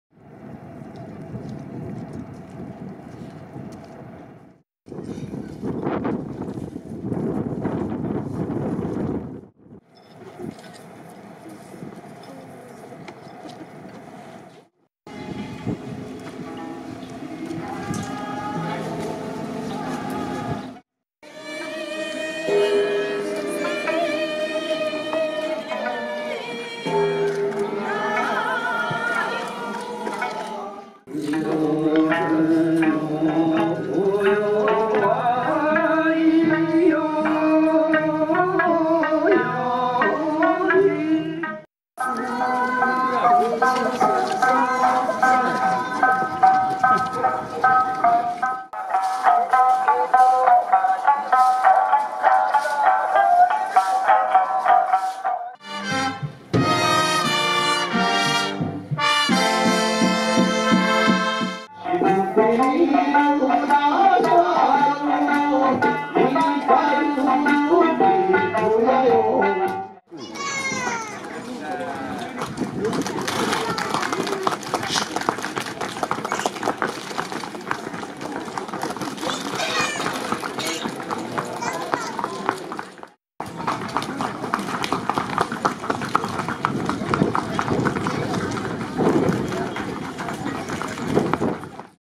平得種子取祭2018
2018年1月8日に開催された、平得種子取祭の様子です。